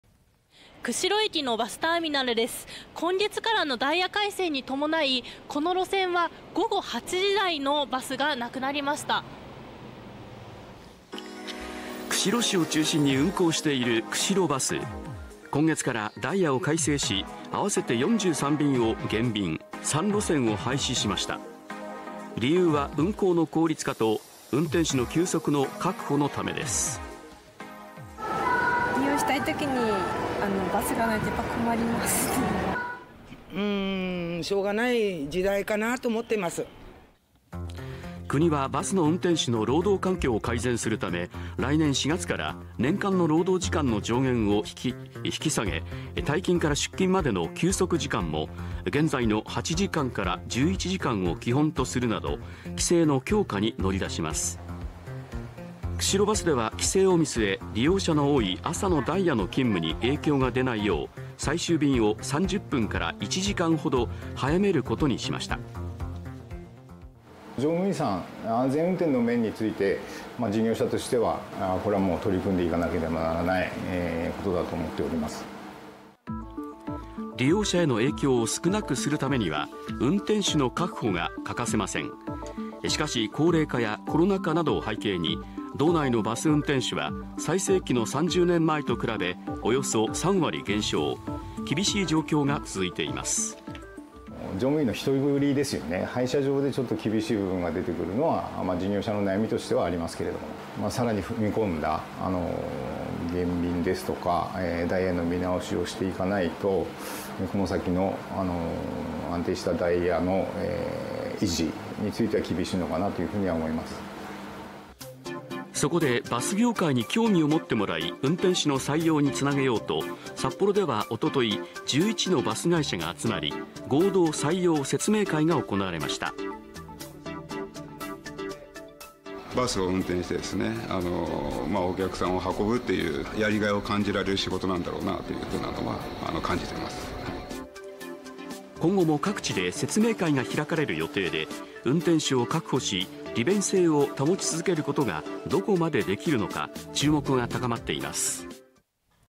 "2024年問題"バスのダイヤに影響 労働規制に備え「減便」「路線廃止」も 運転手の確保不可欠 各地で説明会開催 (23/10/02 19:50)
北海道札幌市では、10月から札幌駅前のバスターミナルが閉鎖され、停留所が各地に分散されましたが、変更はこれだけではありません。2024年4月から運転手の労働規制が強化されるのに備え、北海道内の複数のバス会社が減便などのダイヤの改正に踏み切っています。

　「釧路駅のバスターミナルです。ダイヤ改正に伴い、こちらの路線は午後8時台のバスがなくなりました」（田中 うた乃 記者）

　釧路市を中心に運行しているくしろバス。10月からダイヤを改正し、あわせて43便を減便、3路線を廃止しました。理由は運行の効率化と運転手の休息の確保のためです。

　「利用したい時にバスがないのはやっぱり困りますね。」（バスの利用者）

「（バスの減便は）しょうがない。時代かなと思っています」（バスの利用者）

　国はバスの運転手の労働環境を改善するため、2024年4月から年間の労働時間の上限を引き下げ、退勤から出勤までの休息時間も現在の8時間から11時間を基本とするなど、規制の強化に乗り出します。

　くしろバスでは規制を見据え、利用者が多い朝のダイヤの勤務に影響が出ないよう、最終便を30分～1時間ほど早めることにしました。

　「乗務員の安全面に関しては、事業者として取り組まなければならない」（くしろバス 乗合事業部営業課 石岡 勝巳さん）

　利用者への影響を少なくするためには、運転手の確保が欠かせません。しかし高齢化やコロナ禍などを背景に、北海道内のバス運転手は最盛期の30年前と比べ、約3割減少。厳しい状況が続いています。

　「乗務員の人繰りや配車上で厳しい部分が出てくるのは事業者の悩み。さらに踏み込んだ減便などダイヤの見直しをしていかないと、この先の安定したダイヤの維持は厳しいのかなと思います」（くしろバス 石岡さん）

　そこで、バス業界に興味を持ってもらい運転手の採用につなげようと、札幌では9月30日、11のバス会社が集まり、合同採用説明会が行われました。

　「バスを運転して客を運ぶ、やりがいのある仕事だと感じています」（説明会の参加者）

　今後も道内各地で説明会が開かれる予定で、運転手を確保し、利便性を保ち続けることがどこまでできるのか、注目が高まっています。

　ダイヤ改正に踏み切ったのは、「くしろバス」だけではありません。
「ジェイ・アール北海道バス」も10月1日から、運転手の休息時間確保のため、最終便を繰り上げています。

　さらに「北海道中央バス」は、通常の12月か来春にあわせてダイヤ改正を予定しています。

　運転する側の労働環境の改善はもちろん大切ですが、公共交通機関の利便性とのバランスも大切です。交通弱者の利用が多い路線バスをどう維持していくか。行政も含めた検討も必要になってきそうです。